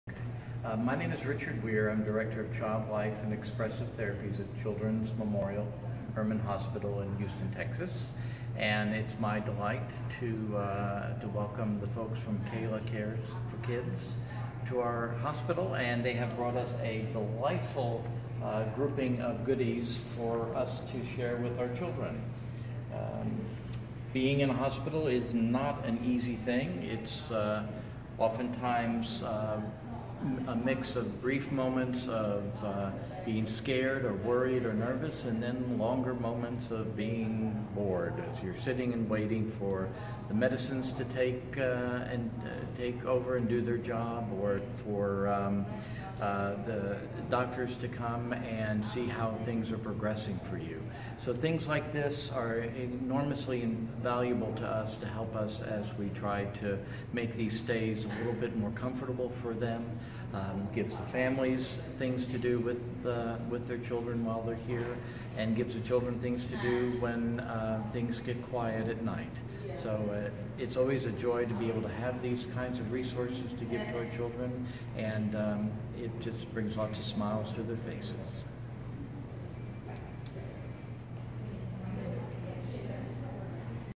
Donation to Children's Memorial Hermann Hospital in Houston
Kayla Cares 4 Kids donated more than 100 items - including DVDs, portable DVD players, CDs, games and crafts to Children's Memorial Hermann Hospital in Houston. Child Life Director Richard Weir explains why this donation means so much to the patients there.